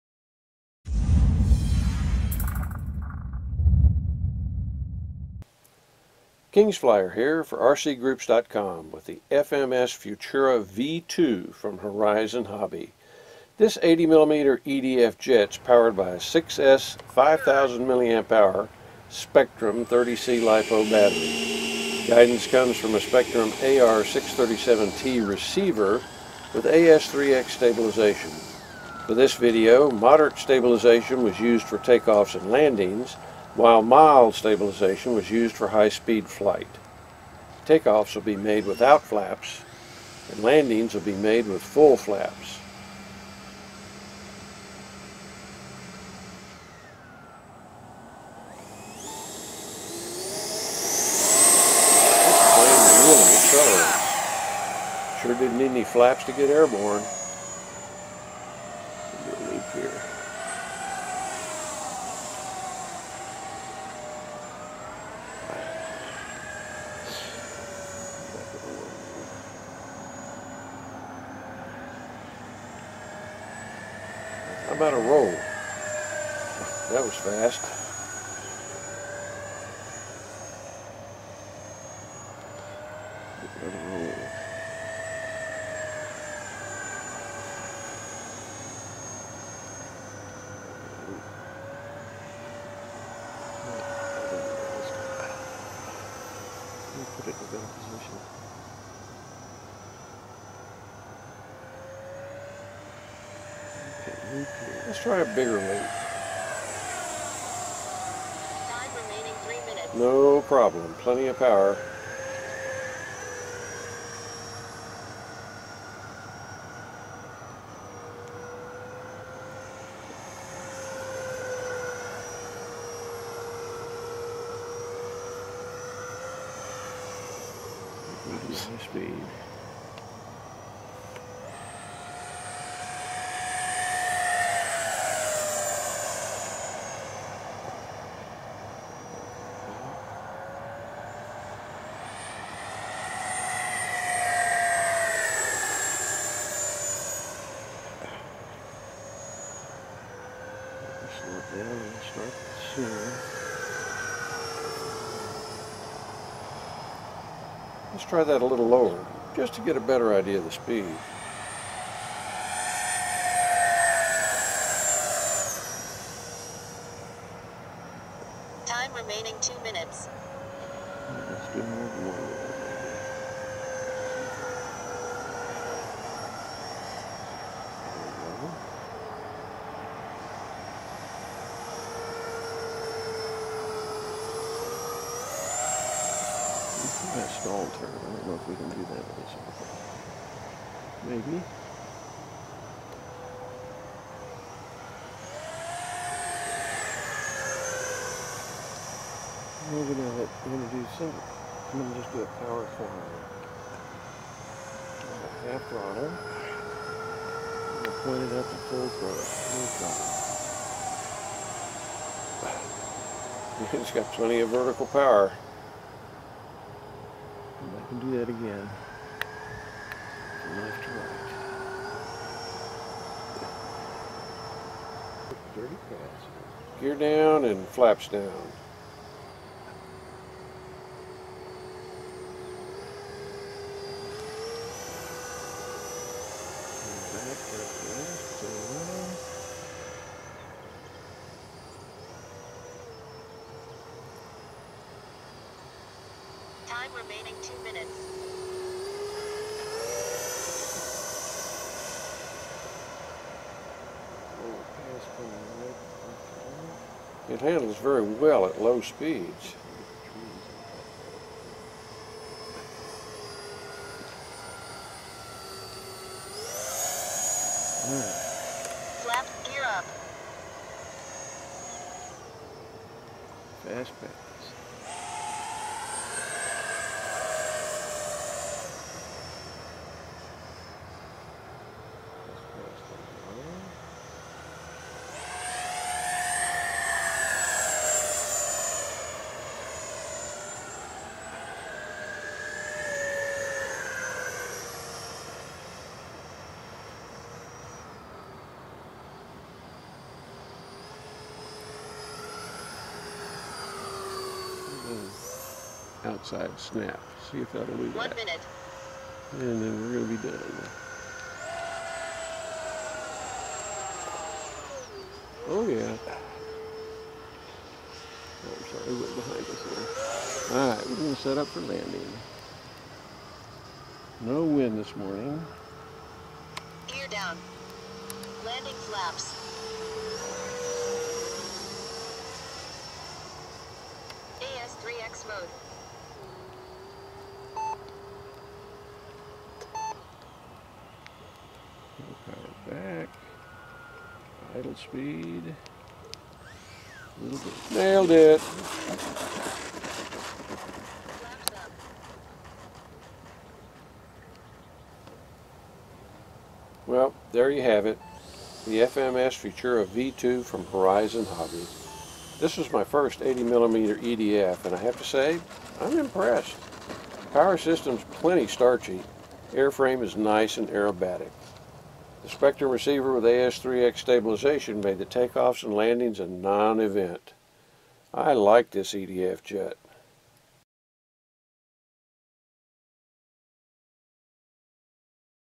FMS Futura V2 from Horizon Hobby - RCGroups.com Review Video
FMS Futura V2 from Horizon Hobby. Flight video and evaluation. This 80mm EDF Jet is FAST and maneuverable. Watch the video and then check out the full Review on RCGroups.com.